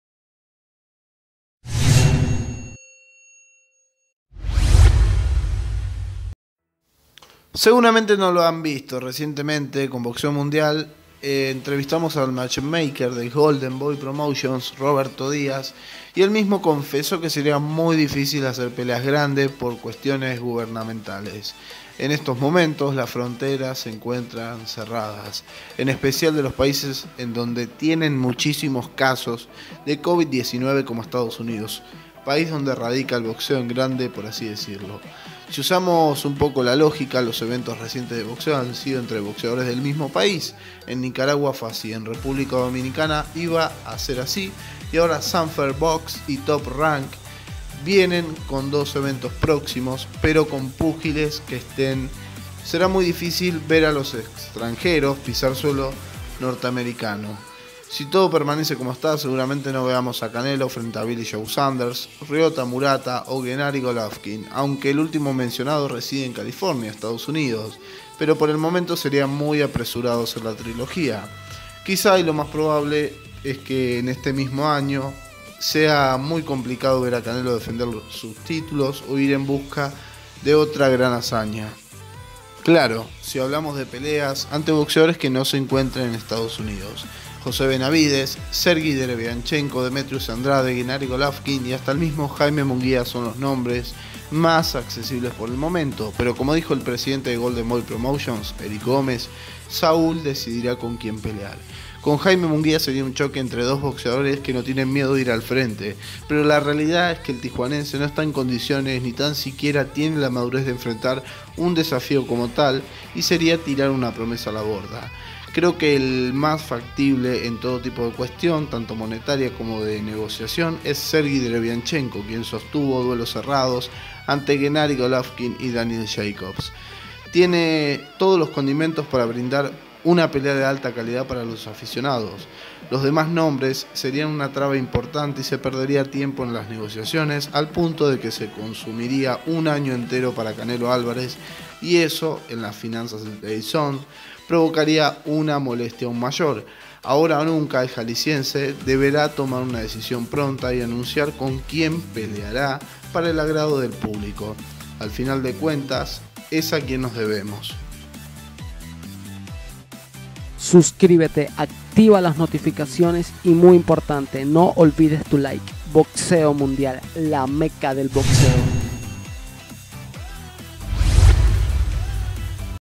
CANELO NO PELEARÁ ESTE MISMO AÑO ANTE BOXEADORES EXTRANJEROS QUE ESTÉN EN SUS PAÍSES.
Seguramente no lo han visto, recientemente BoxeoMundial.com ha entrevistado al matchmaker de Golden Boy Promotions, Roberto Diaz y el mismo confesó que sería muy difícil hacer peleas grandes por cuestiones gubernamentales.